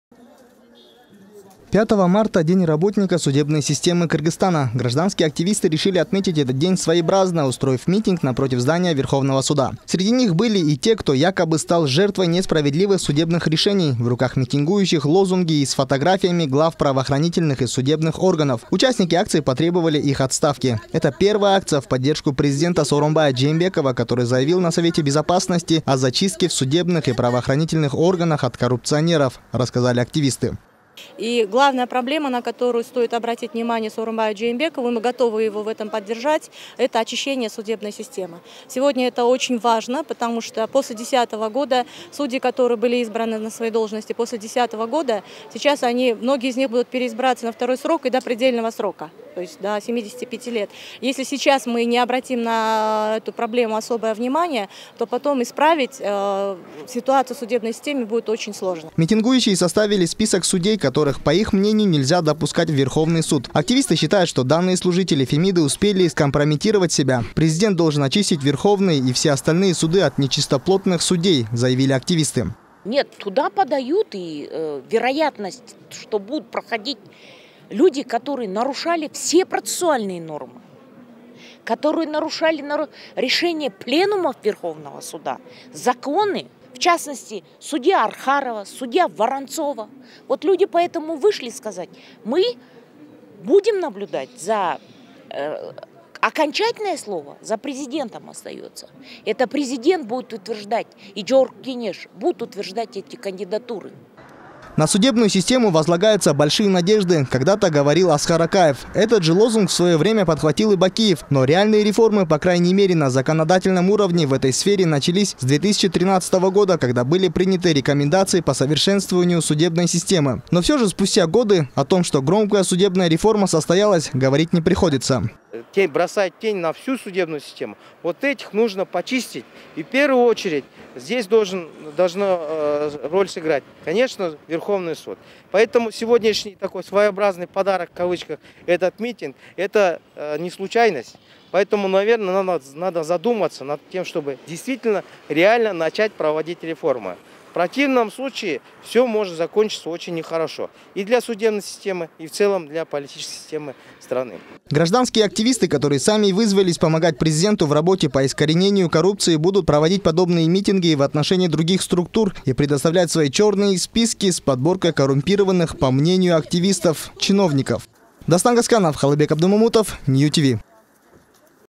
Митинг против "продажных" судов\NewTV
Сегодня у здания Верховного суда прошел митинг. Участники сообщили, что поддерживают действия президента Сооронбая Жээнбекова в борьбе с коррупцией и судебной несправедливостью. Они отмечают, что перестали верить служителям Фемиды и что простые граждане подвергаются гонениям за открыто озвученное мнение.